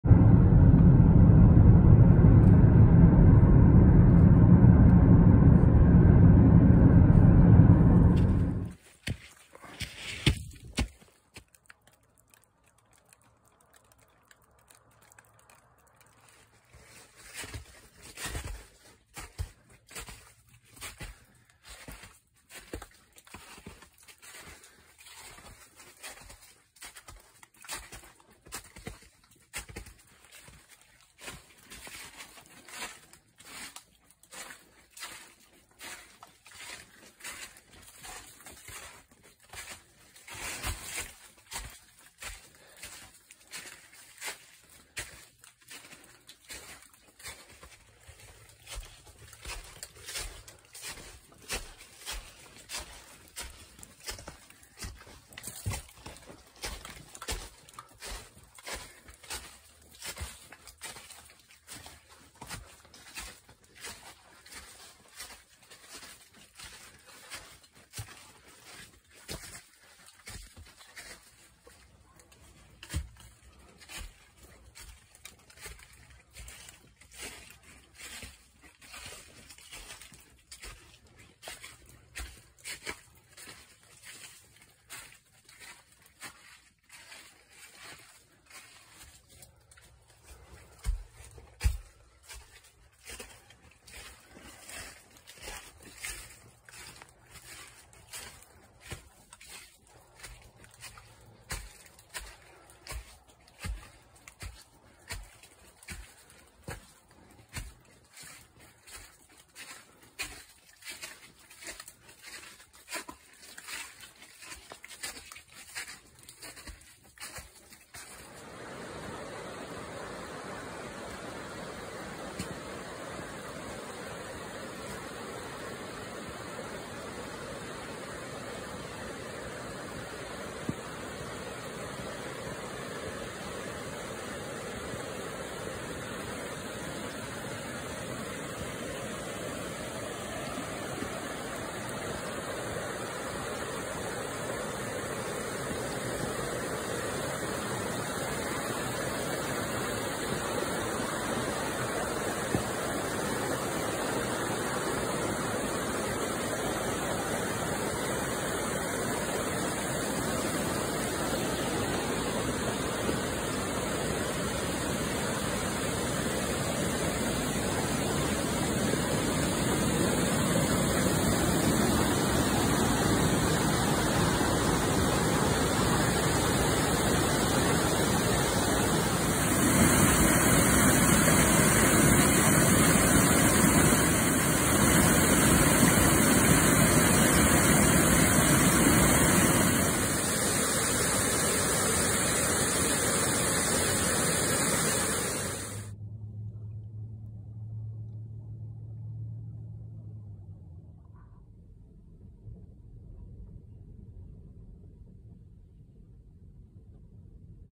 Rock Maze Trail and Swallow Falls State Park (Oakland, MD)
Visit a place like you have never been before!  Journey through the Rock Maze Trail in Oakland, Maryland toward the waterfall at Swallow Falls State Park near Deep Creek Lake.